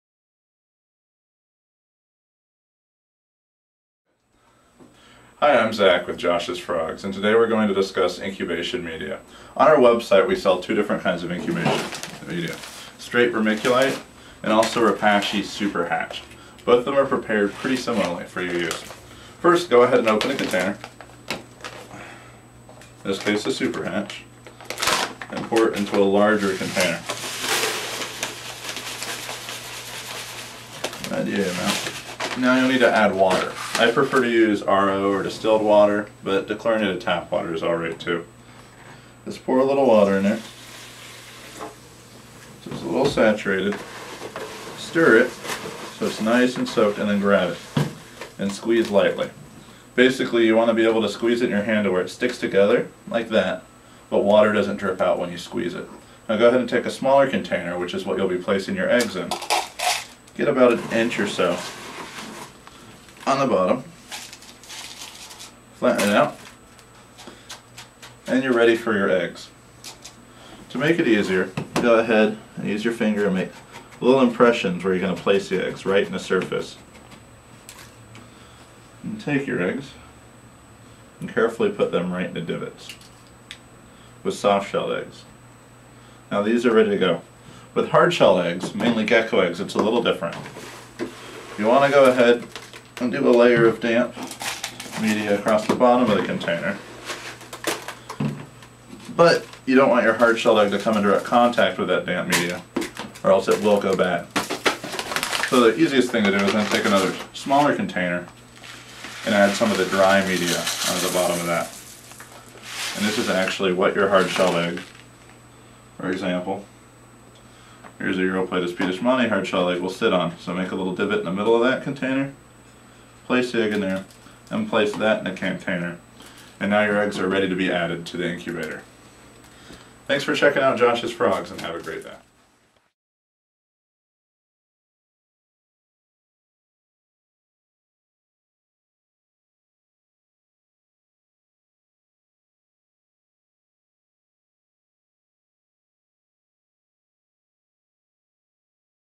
Reptile Egg Incubation
Josh's Frogs has the media and other supplies you need for incubating reptile eggs! Zach will teach you our methods for incubating for soft-shelled eggs and hard-shelled eggs. Let's get hatchin'!